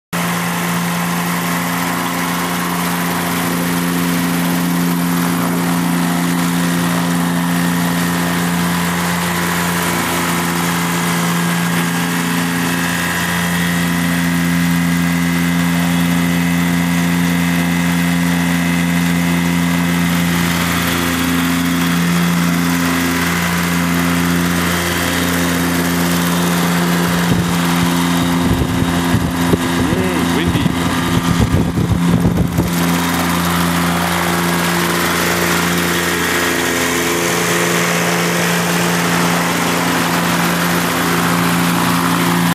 My Gyroplane Romeo Lima run up
Replica MKIV Cambell Cricket without the rotors on wintery engine run up